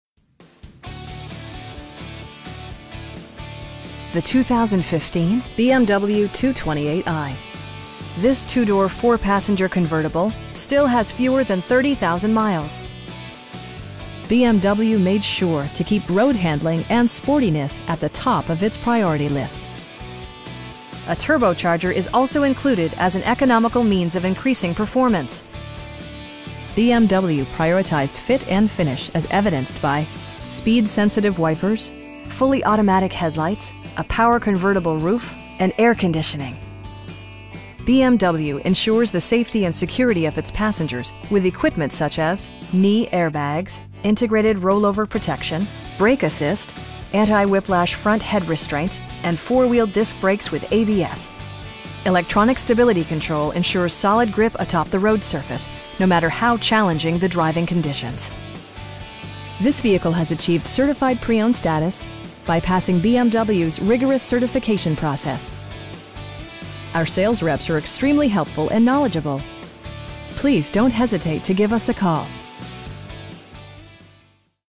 2015 BMW 228i in Wilmington, NC 28403
BMW of Wilmington
4920 New Centre Dr

The 2015 BMW 228i This 2 door, 4 passenger convertible still has fewer than 30,000 miles! BMW made sure to keep road-handling and sportiness at the top of it's priority list. Under the hood you'll find a 4 cylinder engine with more than 200 horsepower, providing a spirited, yet composed ride and drive. A turbocharger is also included as an economical means of increasing performance. BMW prioritized fit and finish as evidenced by: speed sensitive wipers, fully automatic headlights, tilt steering wheel, power door mirrors and heated door mirrors, a power convertible roof, cruise control, and air conditioning. BMW ensures the safety and security of its passengers with equipment such as: dual front impact airbags with occupant sensing airbag, knee airbags, integrated roll-over protection, traction control, brake assist, anti-whiplash front head restraints, a panic alarm, an emergency communication system, and 4 wheel disc brakes with ABS. Electronic stability control ensures solid grip atop the road surface, no matter how challenging the driving conditions. This vehicle has achieved Certified Pre-Owned status, by passing BMW's rigorous certification process. Our sales reps are extremely helpful & knowledgeable. We'd be happy to answer any questions that you may have. Please don't hesitate to give us a call.